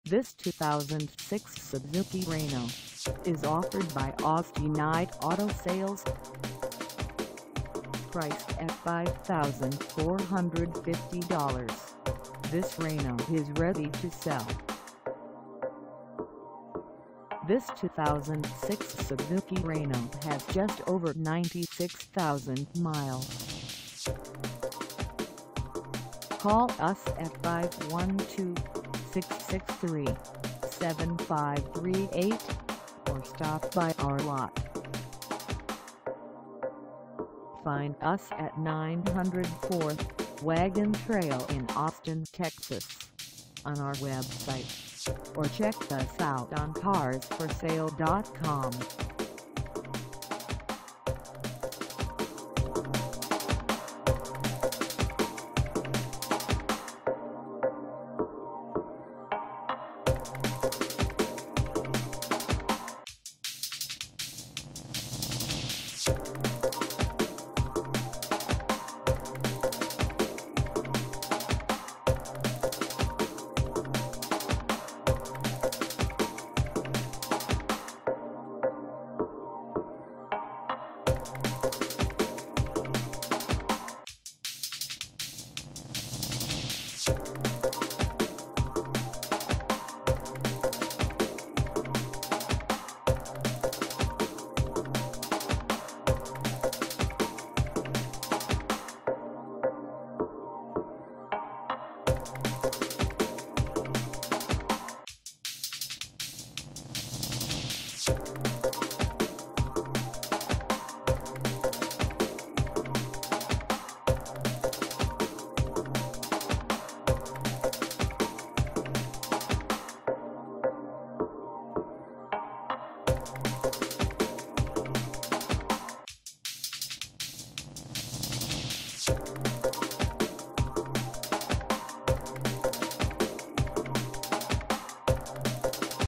2006 Suzuki Reno Base for sale in Austin, TX 78758 at Austin
This 2006 Suzuki Reno Base is for sale in Austin, TX 78758 at Austinite Auto Sales.
Engine: 4-Cylinder L4, 2.0L
Transmission: Automatic
Color: White
Mileage: 96000
Contact Austinite Auto Sales 904 Wagon Trail Austin, TX 78758 to test drive this 2006 Suzuki Reno Base today.